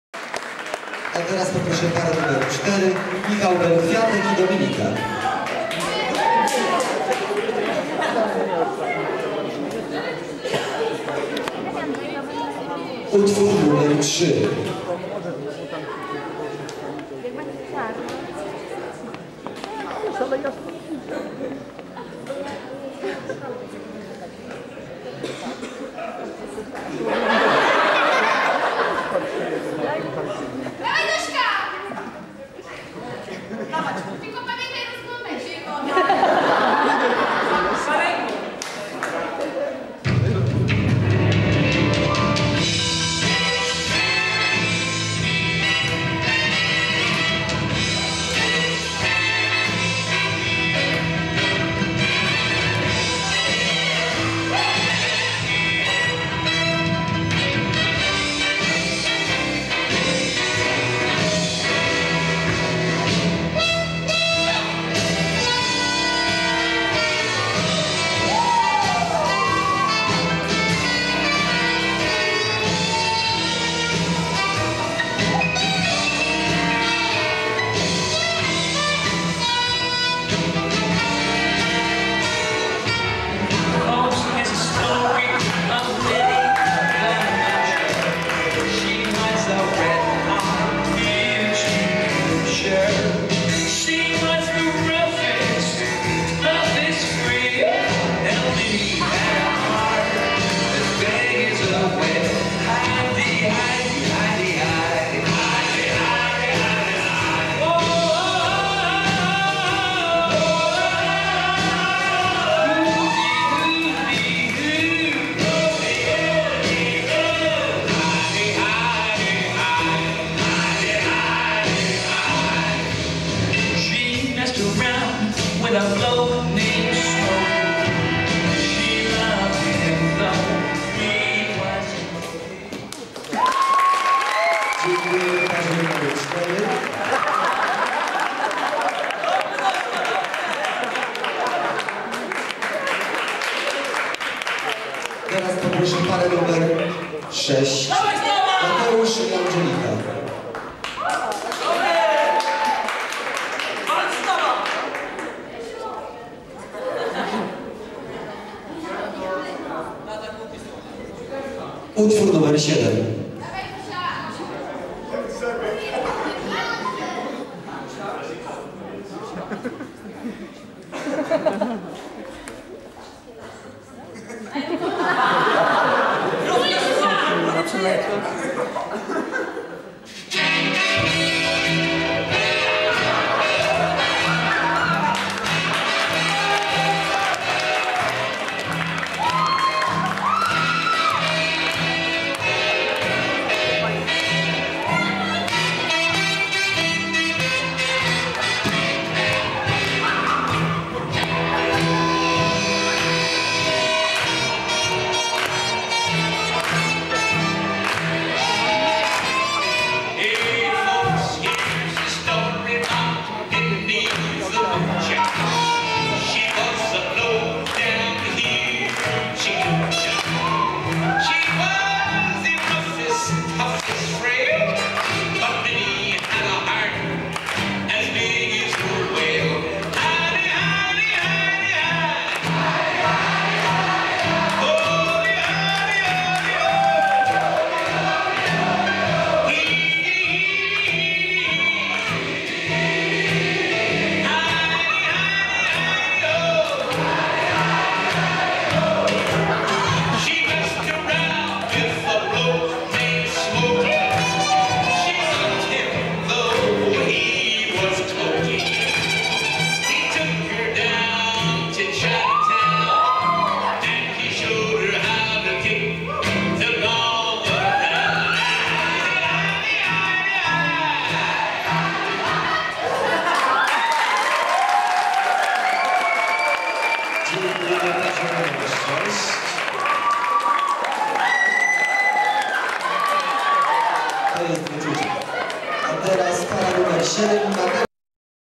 Jack and Jill cz.5
maly final konkurencji... ;) taniec w ktorym partnerka ma zawiazane oczy... boogie woogie na wesolo
tancza: Michal z Doska i Mateusz z Angelika